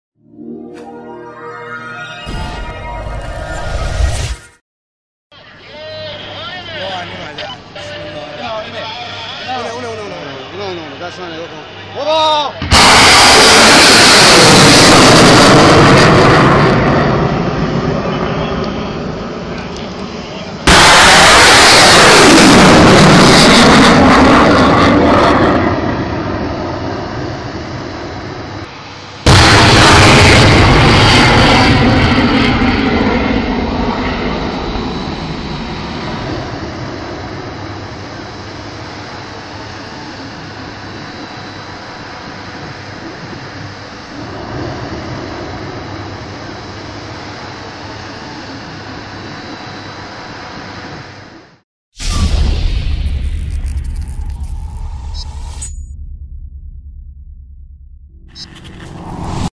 IRAN first lunch of "Shalamcheh" Missiles by Mersad air defence system
IRAN successfully lunched three "Shalamcheh" Missiles by Mersad air defence system